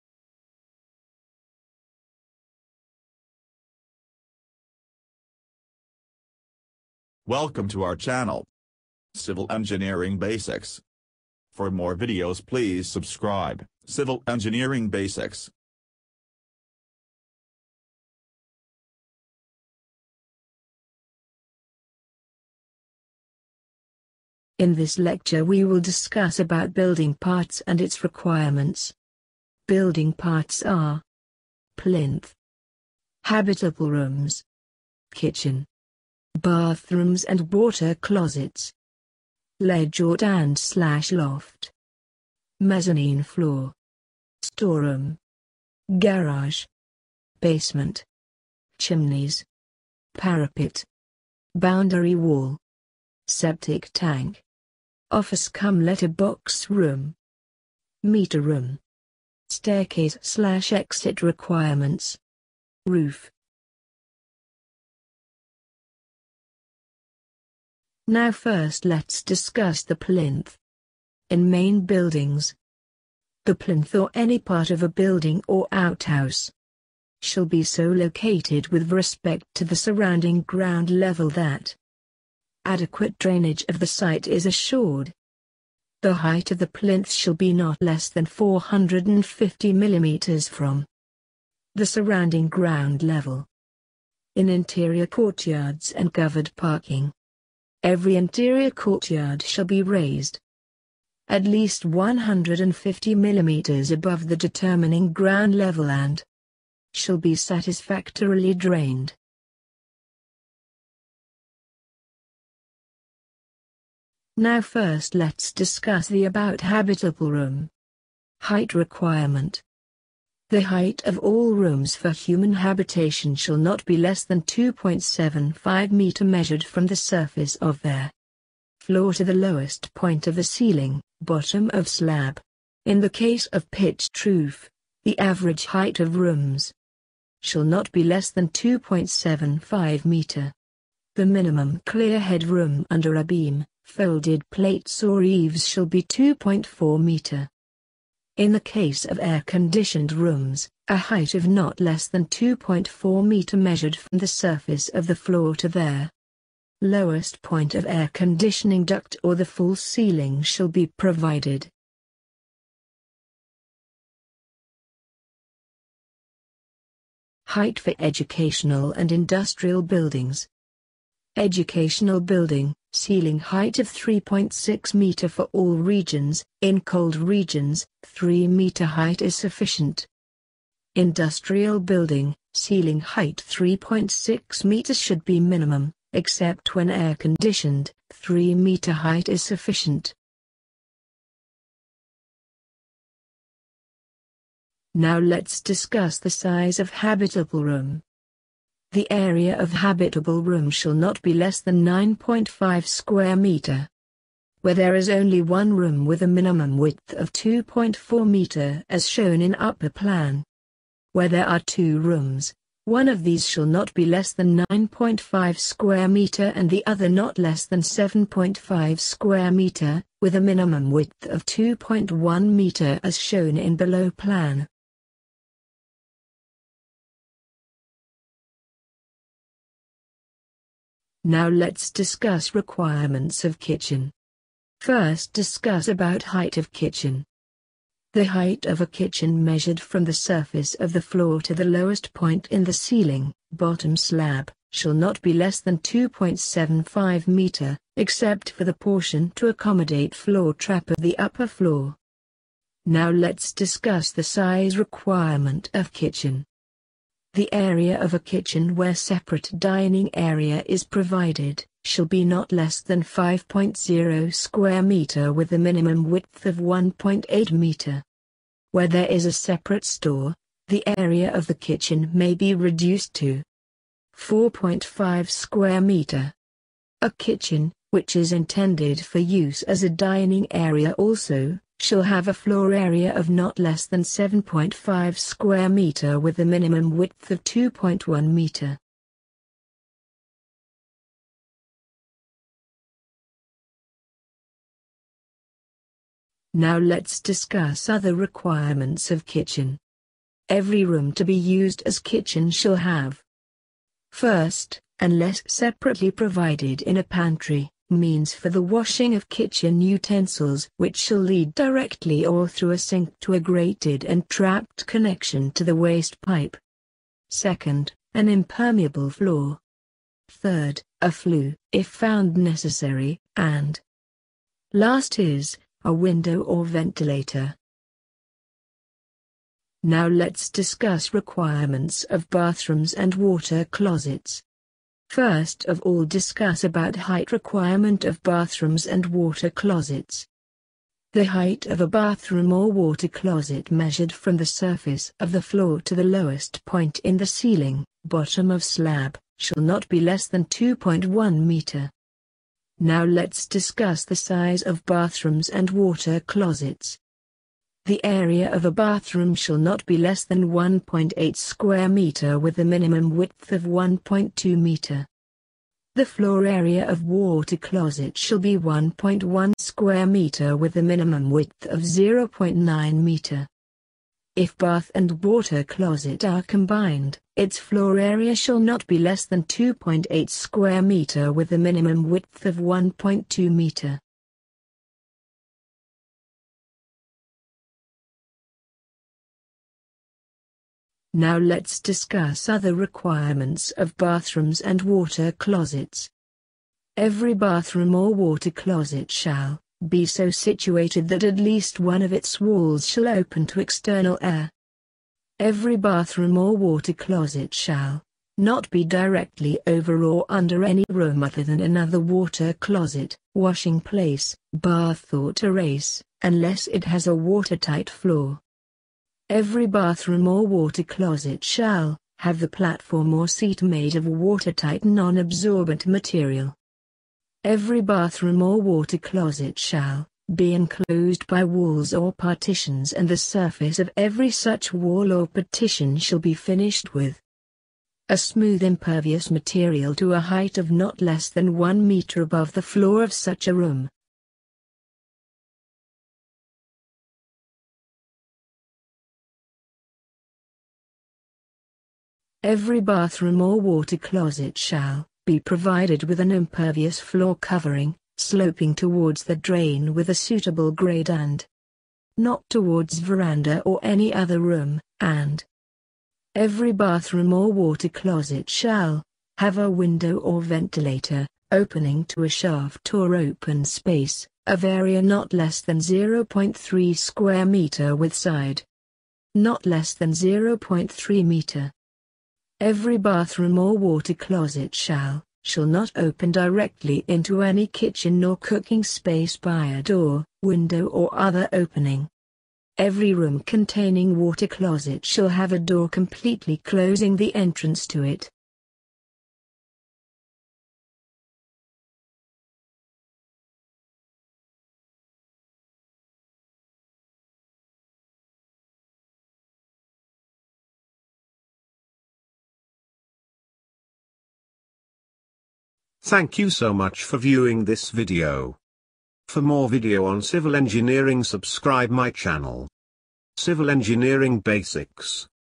REQUIREMENTS OF BUILDING PARTS || AS PER INDIAN STANDARD CODE || PART 1 || BUILDING PLANNING BASICS
THIS VIDEO PROVIDES YOU ALL DATA OF NATIONAL BUILDING CODE IN BRIEF TO UNDERSTAND EASILY AND RAPIDLY.

WHILE BUILDING PLANNING OF RESIDENTIAL BUILDING, SCHOOL BUILDING, INDUSTRIAL BUILDING,ETC. BUILDING PLANNING LAWS IS MOST USEFUL, IN THIS VIDEO ALL OTHER BUILDING COMPONENTS REQUIREMENTS AS PER INDIAN STANDARD IS DISCUSSED, AND EXPLAINED IN DETAIL TO UNDERSTAND IT EASILY.

KEEP WATCHING CIVIL ENGINEERING BASICS FOR MORE INTERESTING CIVIL ENGINEERING VIDEOS.

THE DATA USED IN THIS VIDEO IS TAKEN FROM NATIONAL BUILDING CODE IS:SP-7(2005)

THIS DATA SERVE AS A MODEL FOR ADOPTION  BY PUBLIC WORKS DEPARTMENT AND OTHER GOVERNMENT  CONSTRUCTION DEPARTMENTS, LOCAL BODIES AND OTHER CONSTRUCTION AGENCIES.